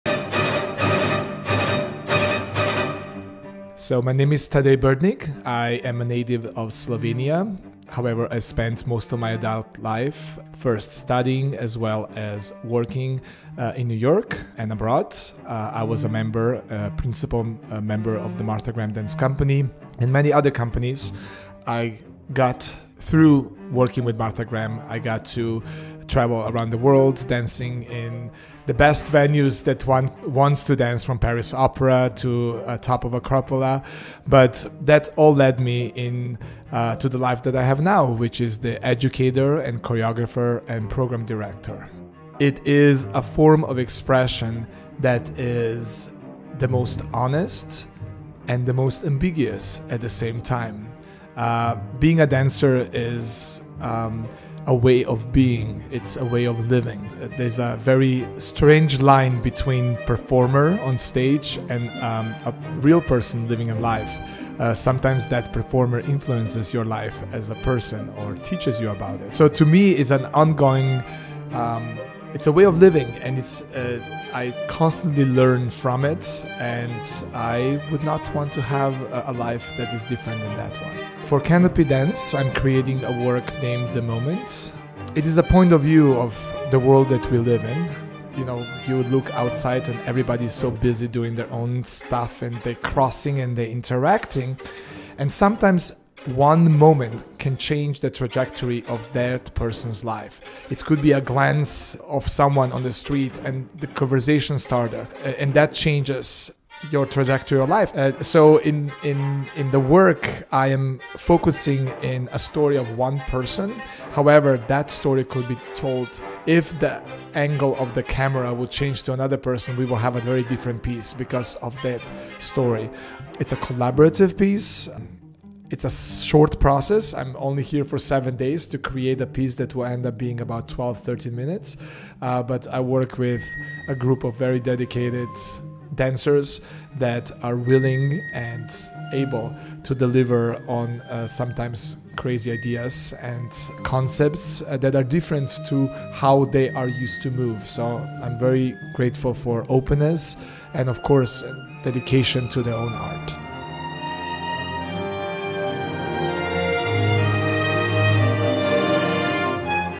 Tadej Brdnik, guest artist Kanopy Dance Company's Shades of Light
Oct 20-22, 2023 at the Overture Center for the Arts